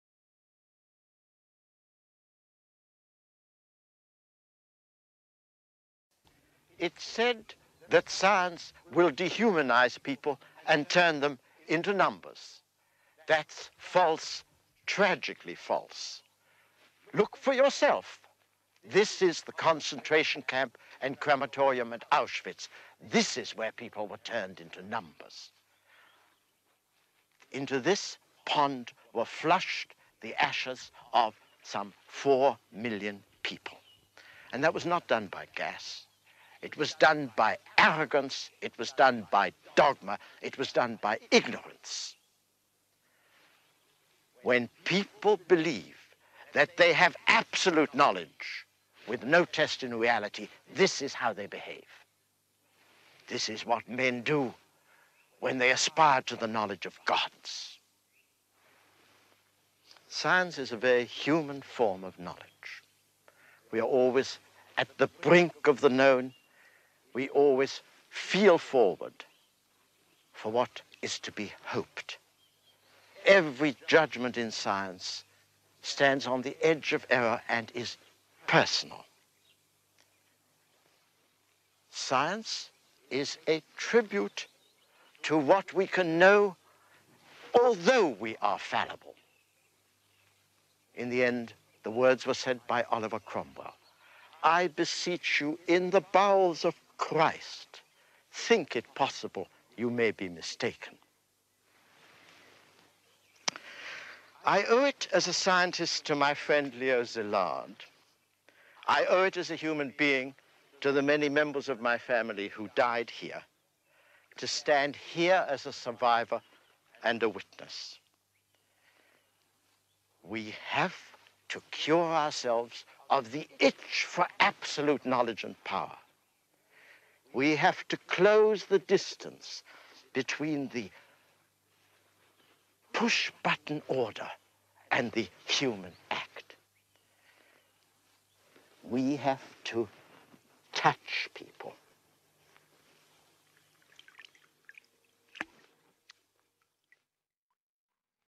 The Danger of Dogma
Jacob Bronowski explains why the pursuit of science is better than seeking absolute knowledge.

With subtlitles, to emphasise the poignancy of his words.